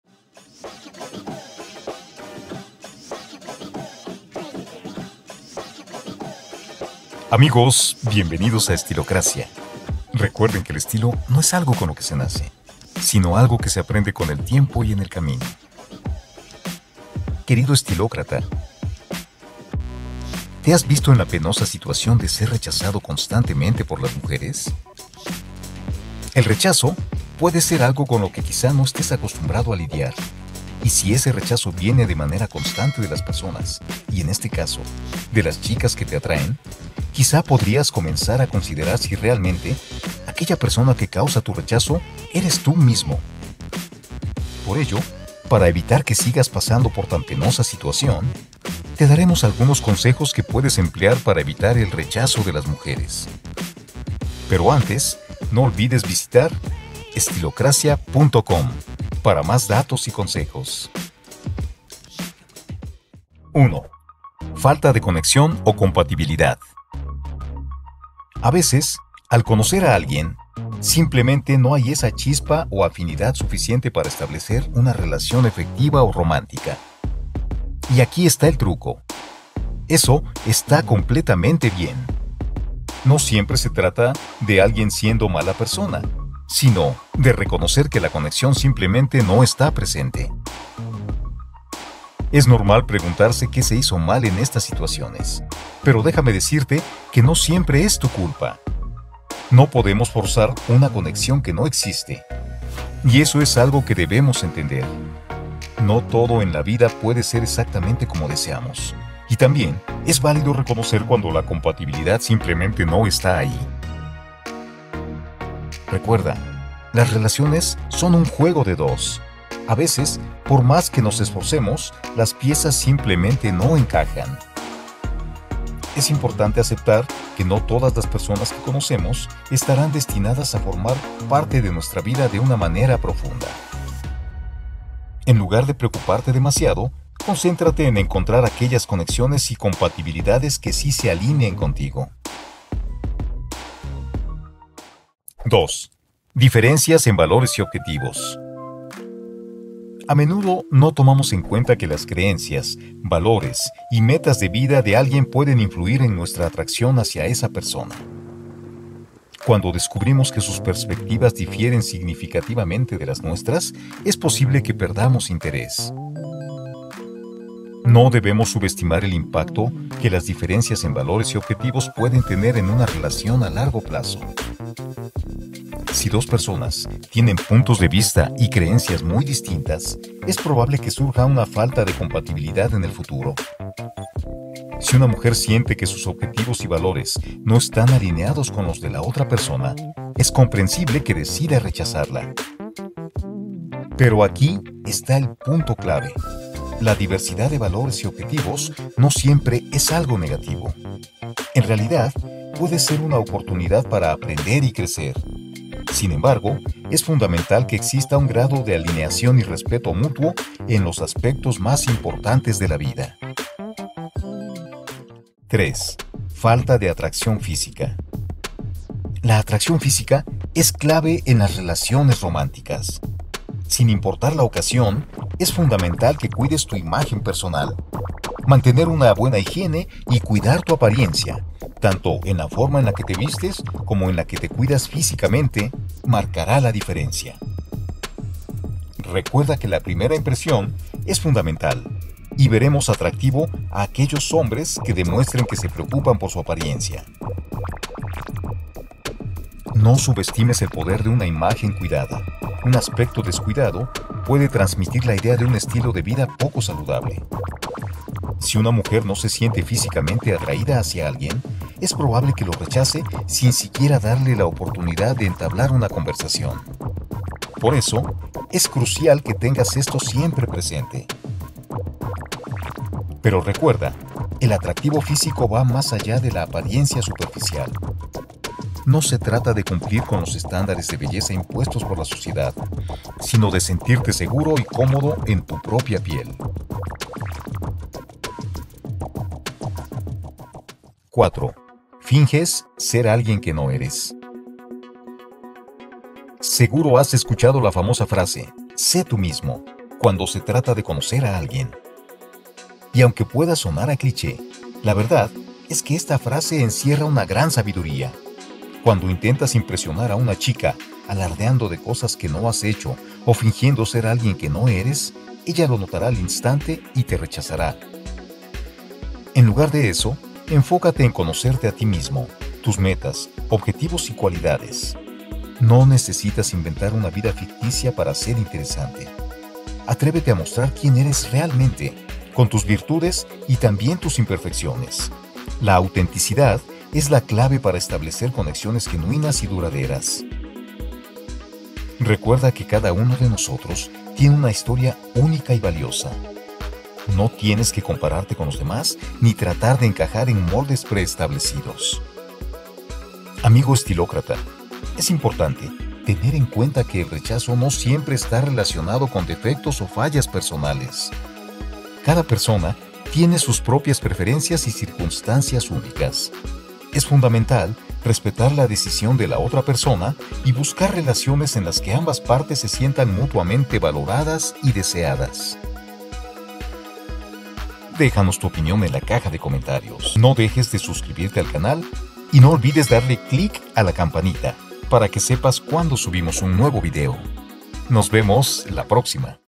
6 trucos PARA NO ser RECHAZADO por LAS chicas 👩🏻‍🦰 ❌!!!
👩🏻‍🦰 LAS CHICAS te RECHAZAN todo el tiempo 😢? TOMA nota DE esto!!

00:00 Introducción
01:02 Falta de conexión
02:25 Diferencias
03:35 Falta de atracción física
04:50 Finges ser alguien que no eres
05:53 Despedida

Visita nuestro sitio asociado de Amazon para productos de tu interés: